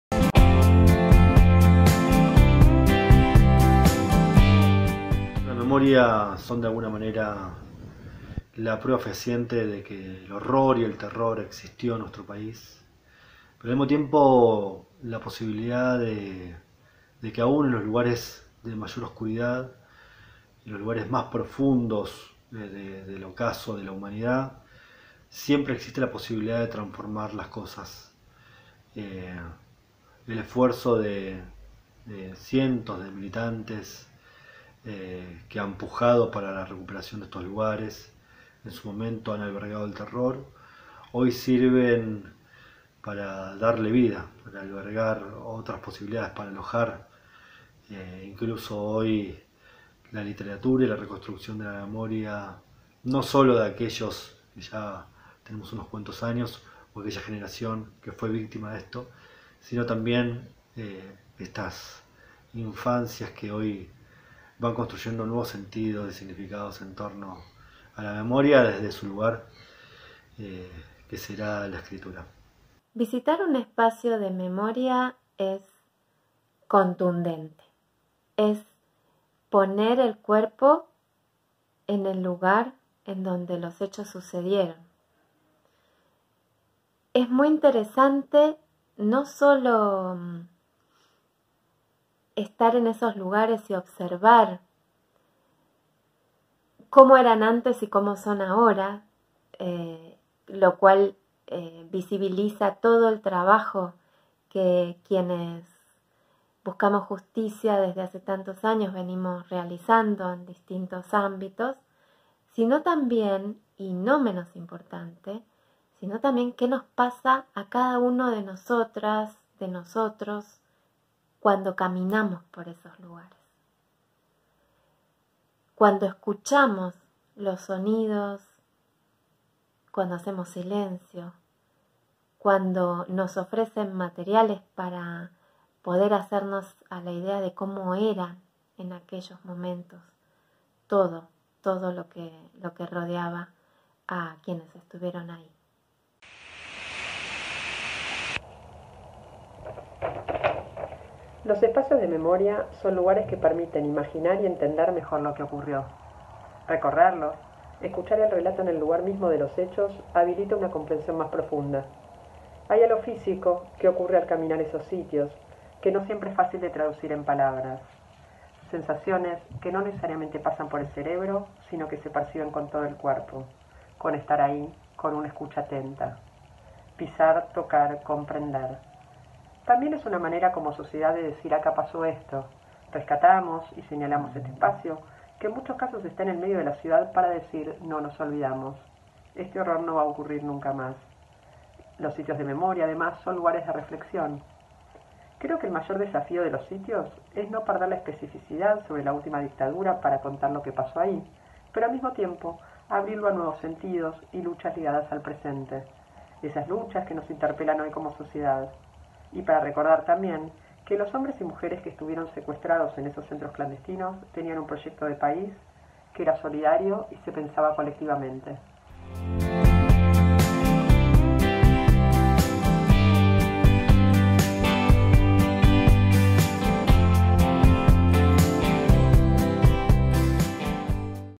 Conocer los Espacios para la Memoria, parte 1 | La Memoria no es un cuento II
✍️ En el marco del 2° certamen literario "La Memoria no es un Cuento" nos preguntamos:
¿POR QUÉ ES IMPORTANTE CONOCER LOS ESPACIOS PARA LA MEMORIA?

📚 Invitamos a quienes son parte del jurado y especialistas en políticas sociales y derechos humanos a que nos den sus opiniones. Escuchalos.